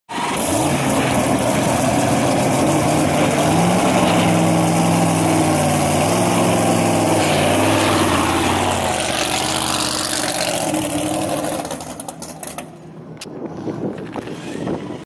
"SLAMMED HEMI '57 CHEVY WAGON" @ DRAG WEEK
SHOT BY RACEBARN MOTORSPORTS @ "ATLANTA DRAGWAY".